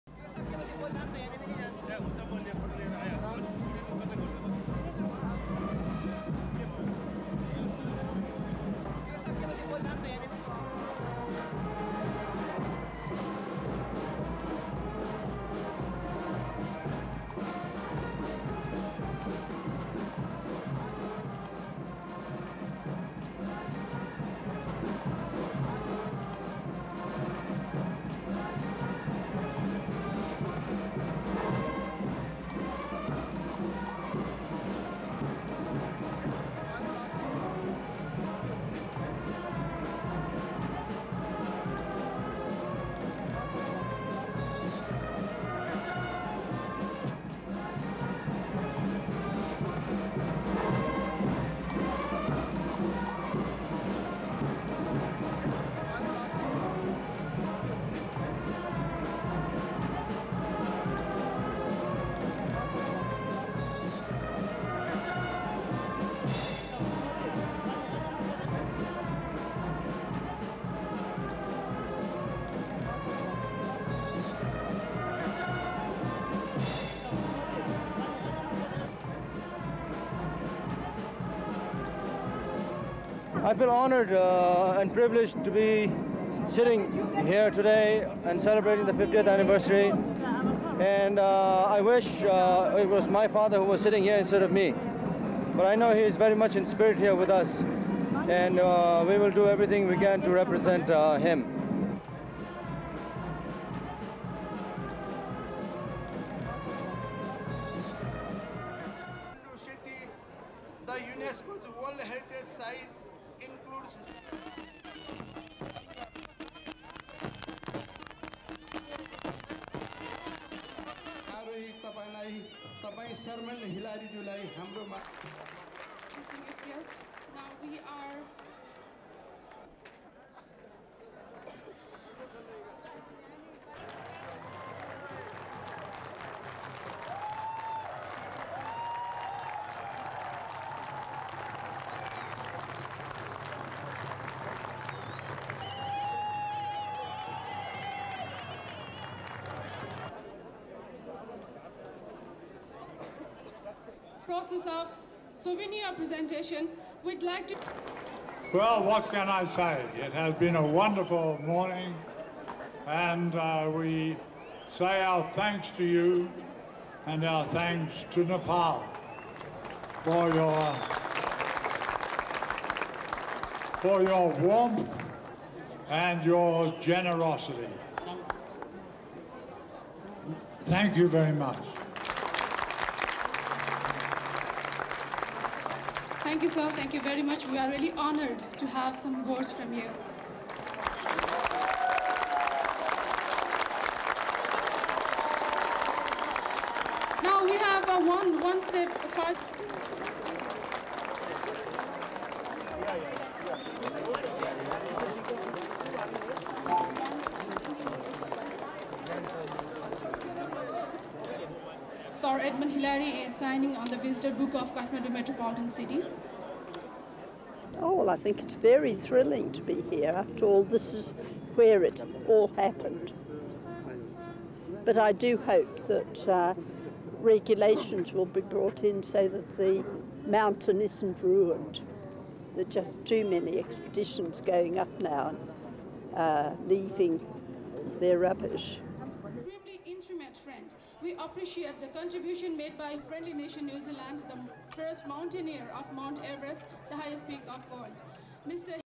Sir Edmund Hillary joins Everest mountaineers in carriage procession
1. Mid shot Sir Edmund Hillary and his wife, June in carriage with Dyalzen Sherpa, the only surviving Sherpa from the 1953 expedition, at start of procession
2. Close up Hillary
3. Cutaway horses
4. Wide shot shot procession Sir Edmund Hillary and his wife, June waving, Gurkha army band playing
5. Schoolchildren in procession
6. Trombone
7. Various procession
8. SOUNDBITE: (English) Jamlin Norgay, son of Tenzing Norgay:
"I feel honoured and privileged to be sitting here today celebrating the 50th anniversary and I wish it was my father sitting here instead of me. But I know he is very much in spirit here with us and we will do everything we can to represent him."
9. Two shots schoolchildren carrying placards saying "Summiteers you are great."
10. Cutaway police
11. Hillary and wife sitting at ceremony with Junko Tabei and Dyalzen Sherpa, the only surviving Sherpa from the 1953 expedition
12. People watching from windows
13. SOUNDBITE: (English) Sir Edmund Hillary:
"What can I say. It's been a wonderful morning and we say our thanks to you and our thanks to Nepal for your warmth and your generosity. Thank you very much."
14. Hillary being presented with presented with framed picture- gift from Nepal Mountaineering Association
15. Wide above
16. Cutaway crowd
17. Cutaway of Reinhold Messner of Italy, the first to climb it without bottled oxygen
18.  SOUNDBITE: (English) Sir Edmund Hillary:
19. Crowd
20. Jamlin Norgay, son of Tenzing Norgay being presented with framed picture- gift from Nepal Mountaineering Association
21. Hilary signing Katmandu City's visitor book
22. SOUNDBITE: (English) June Carlisle, Sir Edmund Hillary's sister:
"I think it is very thrilling to be here after all this is where it all happened but I do hope that regulations will be brought in so the mountain isn't ruined. There are just too many expeditions going up now leaving their rubbish."
23. Cutaway onlookers
24. Wide shot ceremony

STORYLINE:

Sir Edmund Hillary and a Sherpa from the 1953 Mount Everest expedition led a colourful procession of hundreds of climbers and their fans through the Nepalese capital on Tuesday to celebrate the 50th anniversary of the conquest of the world's tallest mountain.

Gyalzen Sherpa, 85, one of three surviving Sherpas from the first successful Everest expedition, climbed into a horse-drawn carriage with Hillary, 83, and his wife, June, and placed yellow scarves around their necks.

They set off at the head of a parade of carriages bearing other famous Everest mountaineers, while a Gurkha army band played bagpipes and drums, and children waved flags and signs.

Jamlin Norgay, son of Tenzing Norgay, rode the second carriage, with Junko Tabei from Japan, the first woman the climb Everest.

The procession paused in Katmandu's central Durbar Square, where the Nepal Mountaineering Association and city officials handed scarves, books, wooden carvings and photographs to the famous climbers, also including Reinhold
Messner, the first person to climb Everest without bottled oxygen.

More than 1,300 climbers have scaled the summit in the past 50 years.

Two-thirds of them are believed to be still alive, although only about 100 have come to Nepal's weeklong
celebration.

Organisers of the celebrations said they were expecting between 200-300 climbers to join the festivities.

To mark the anniversary, a record number of people are climbing Everest this month.